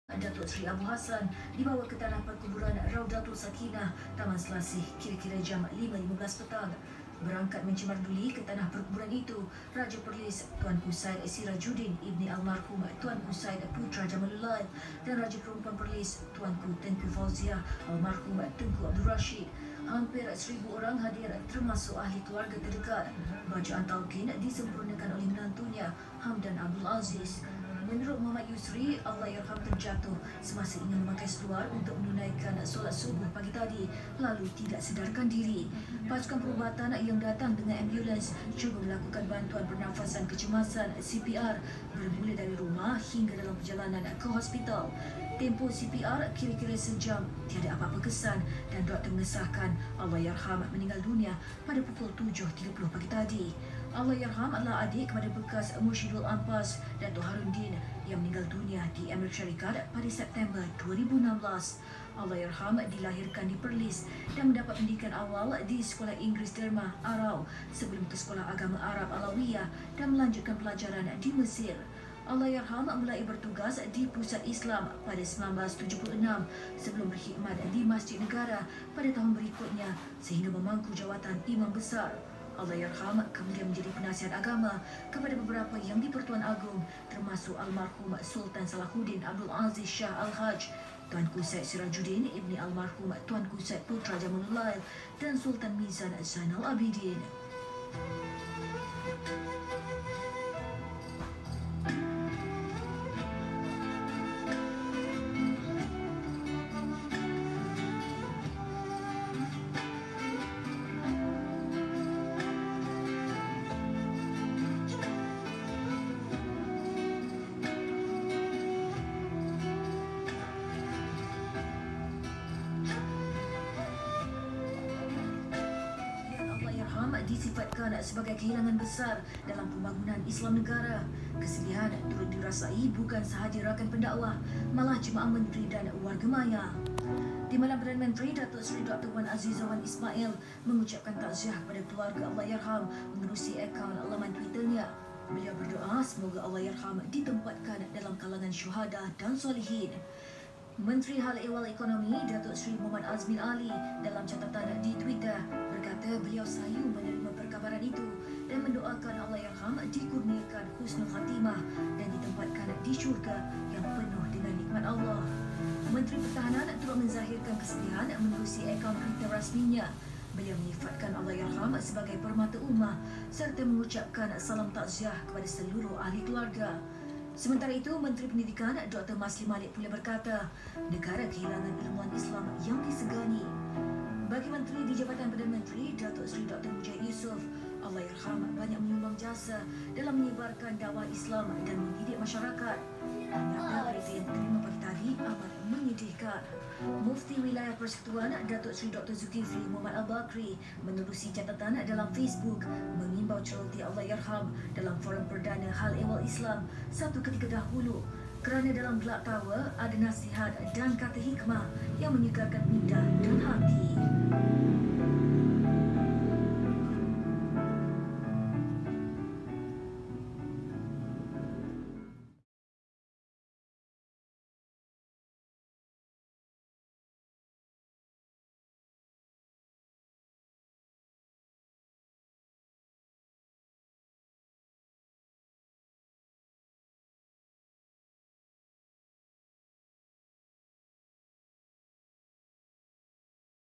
Suasana pilu mengiringi perjalanan terakhir Almarhum Dato' Seri Ustaz Abu Hassan Din Al-Hafiz
Suasana pilu mengiringi perjalanan terakhir Almarhum Dato' Seri Ustaz Abu Hassan Din Al-Hafiz. Semoga Allah mengampuni dan menempatkan arwah di kalangan orang2 beriman.

اللَّهُـمَّ اغْفِـرْ لَهُ ، وَارْحَمْـهُ ، وَعَافِهِ ، وَاعْفُ عَنْـهُ ، وَأَكْـرِمْ نُزُلَـهُ ، وَوَسِّـعْ مُدْخَـلَهُ ، وَاغْسِلْـهُ بِالْمَـاءِ وَالثَّـلْجِ وَالْبَـرَدِ ، وَنَقِّـهِ مِنَ الْخَطَـايَا كَمَا نَـقَّيْتَ الـثَّوْبَ الأَبْيَـضَ مِنَ الدَّنَـسِ،وَأَبْـدِلْهُ دَارًا خَـيْرًا مِنْ دَارِهِ ، وَأَهْلًا خَـيْرًا مِنْ أَهْلِـهِ ، وَزَوْجًا خَـيْرًا مِنْ زَوْجِهِ ، وَأَدْخِـلْهُ الْجَـنَّةَ ، وَأَعِـذْهُ مِنْ عَذَابِ الْقَـبْرِ وَعَذَابِ النَّـارِ